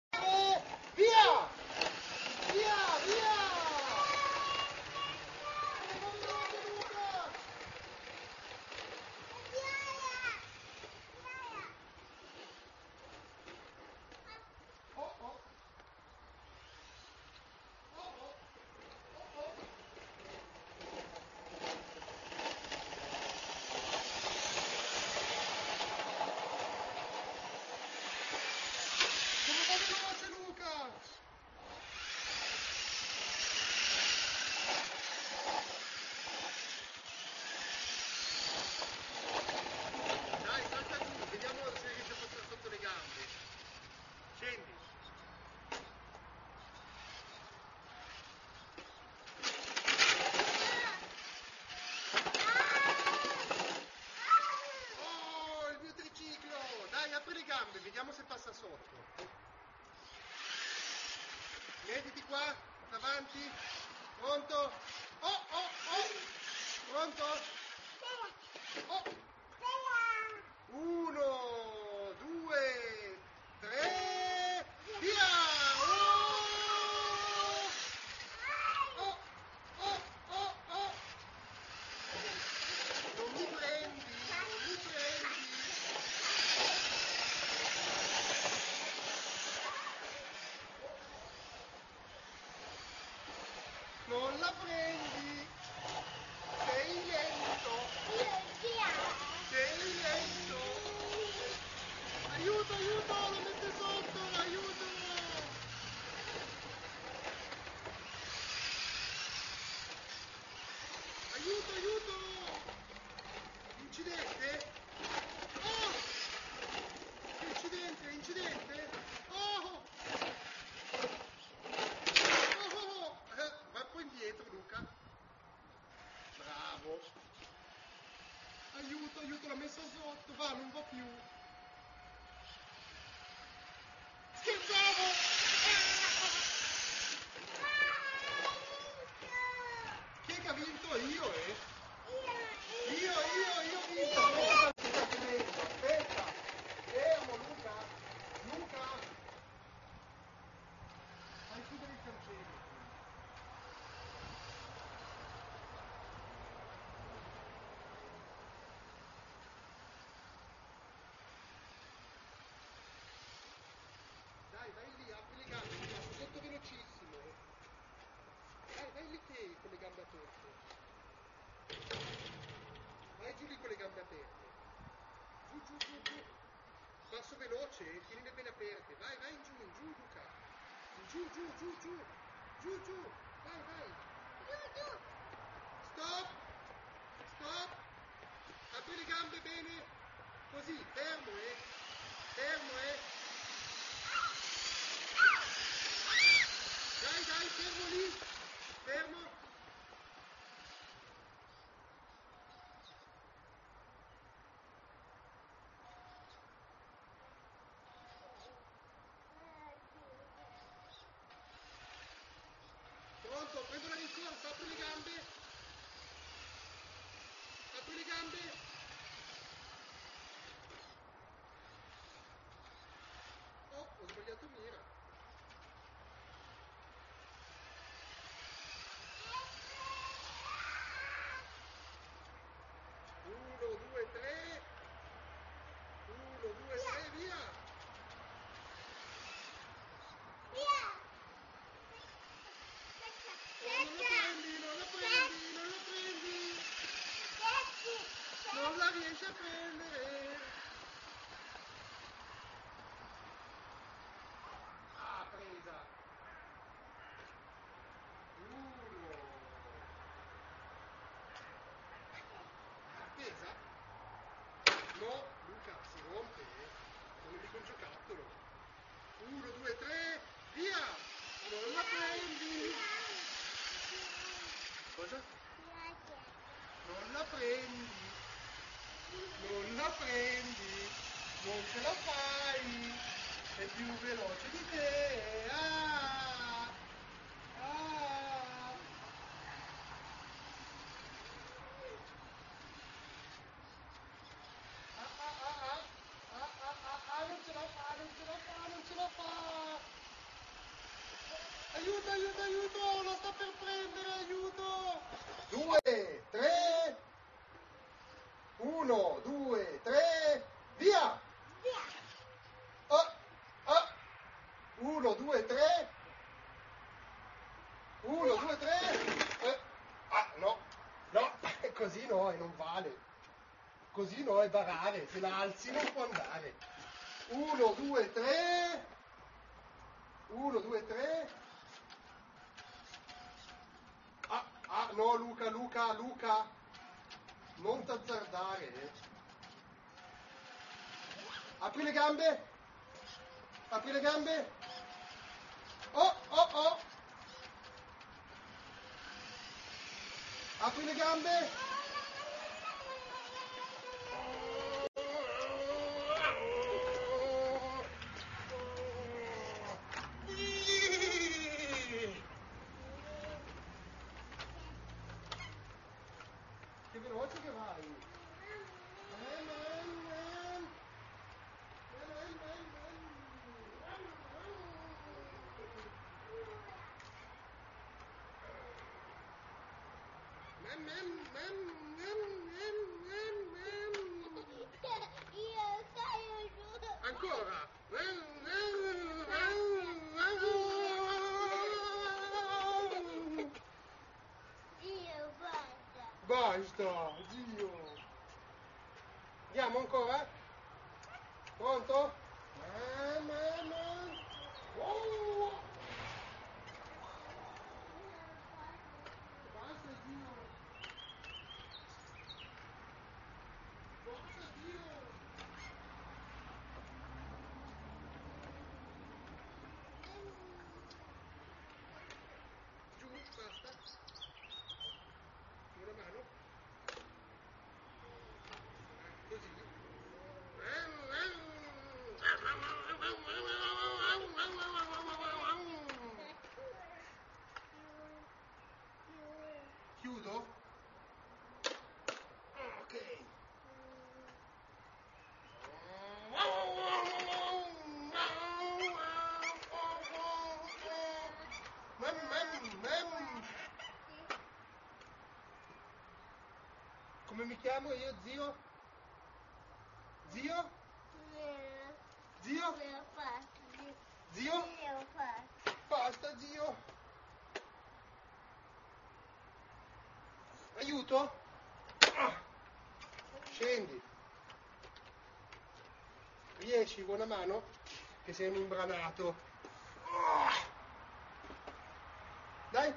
Luca a casa bosisio, giochi con macchina RC e triciclo
Il mio nipotino mentre giochiamo in cortile
con il triciclo e la macchina radiocomandata...